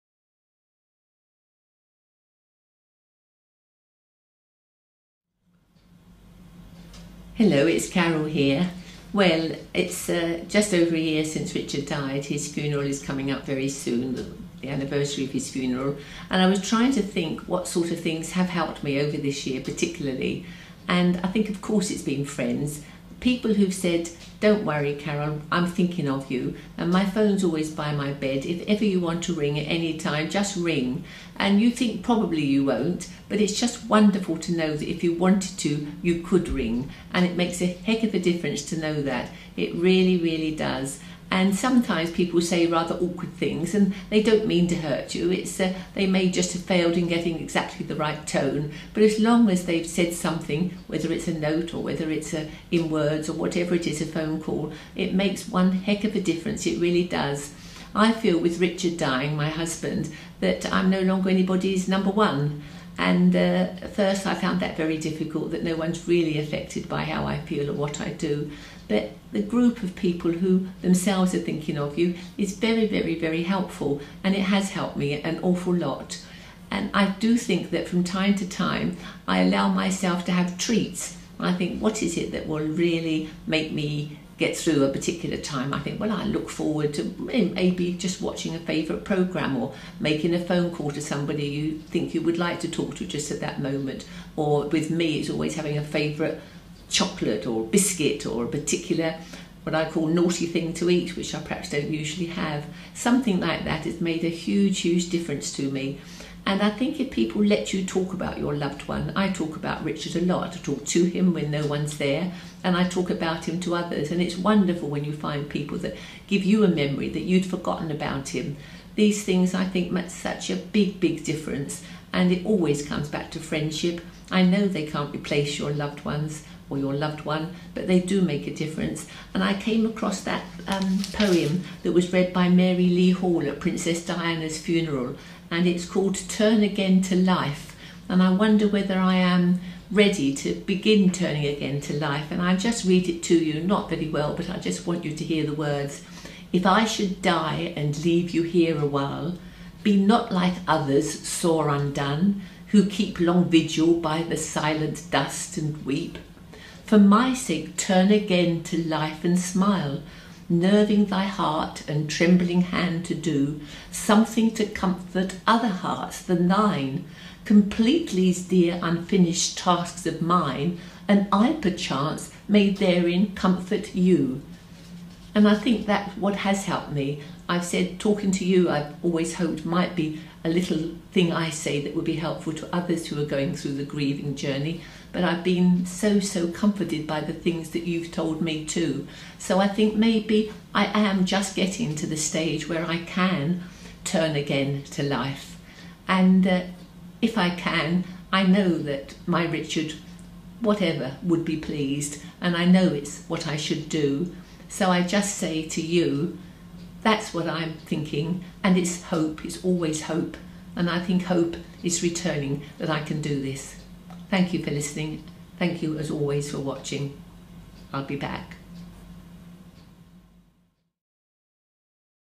Carole Stone Episode 9 Grief and Taking Life in Your Stride
Hello, I’m Carole Stone.   Last November my husband, the former BBC Panorama and ITN broadcaster Richard Lindley, died following a lorry accident.   Richard had lived with Alzheimer’s for three years and was very frail, but thankfully he always knew me. I have spent a year trying to accept his death and want to share my journey with you, in the hope something I say could be helpful and that I may learn from you.

I am aiming to post a new short video each Friday and Monday for the next few weeks.